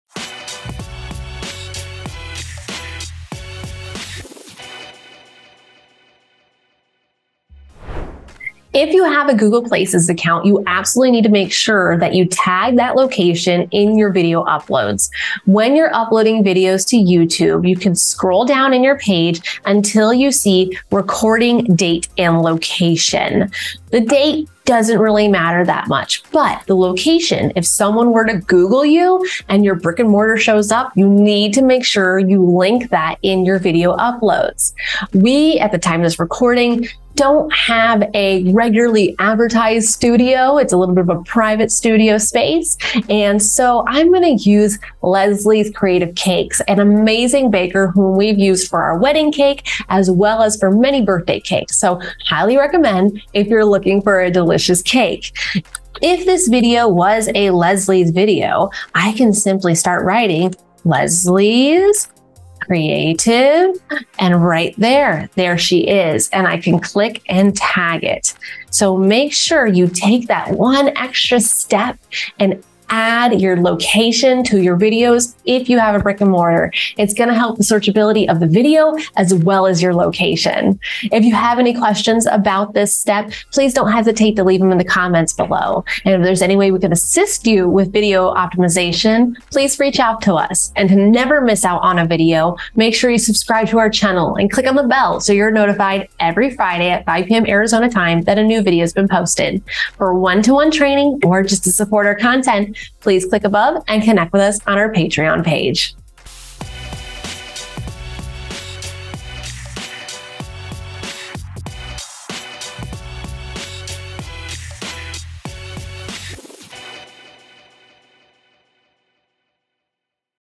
How to Search Engine Optimize Videos: Location! Boost Businesses by Tagging Filming Locations!
If you have a Google Places account,

you absolutely need to make sure that you tag that location in your video uploads.

When you're uploading videos to YouTube, you can scroll down in your page until you

see Recording Date and Location.

The date doesn't really matter that much, but the location.

 If someone were to Google you and your brick and mortar shows up,

you need to make sure you link that in your video uploads.

We at the time this recording don't have a regularly advertised studio.

It's a little bit of a private studio space.

And so I'm going to use Leslie's Creative Cakes, an amazing Baker,

who we've used for our wedding cake as well as for many birthday cakes.

So I highly recommend if you're looking for a delicious cake.

If this video was a Leslie's video,

I can simply start writing Leslie's creative.

And right there. There she is.

And I can click and tag it.

So make sure you take that one extra step and add your location to your videos.

 If you have a brick and mortar, it's going to help the search ability of the video as well as your location!